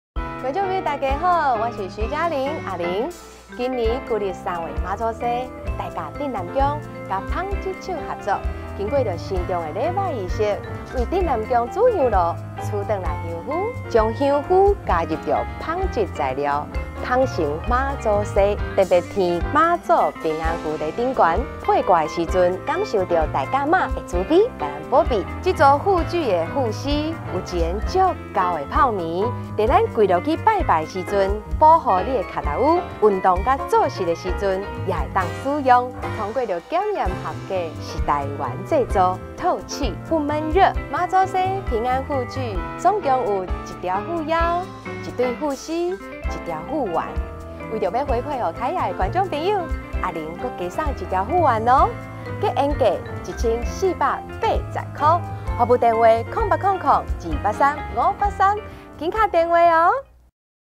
【拜蝦咪】媽祖紗平安護具組 獨家贈送：透氣香灰護腕1個
◆媽祖紗由來
由台中大甲鎮瀾宮與紡織綜合研究所共同合作研發，將信仰與科技結合，創造出媽祖紗。
媽祖紗取自大甲鎮瀾宮的主香爐香灰，經過慎重的儀式取出，透過篩檢去除雜質及較粗顆粒，經過研磨、奈米分散及混練造粒的過程製作成含香灰母粒，將母粒在複合紡紗系統，經精密計量後添加在芯鞘型複合纖維內，再將此纖維加工成媽祖紗。
『天上聖母–媽祖』一直是台灣民眾的重要信仰。此次文化創新產品將「護具」與「媽祖紗」結合，一方面護具能保健身體，一方面融合了宗教意義，信眾穿戴在身上，使廣大民眾感受到媽祖的慈悲與庇祐。